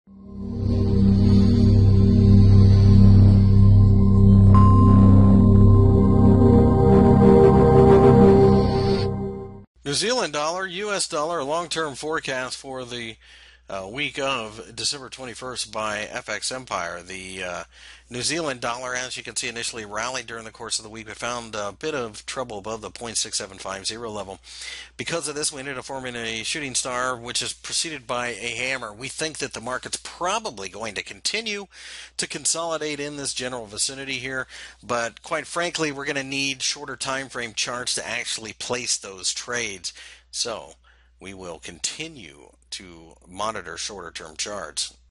NZD/USD Forecast for the week of December 21 2015, Technical Analysis
- December 21 2015 currency weekly technical analysis for the NZD/USD pair.Find more information about Forex News Forex Technical Analysis Forex Fundamental Analysis and Forex Brokers on FX Empire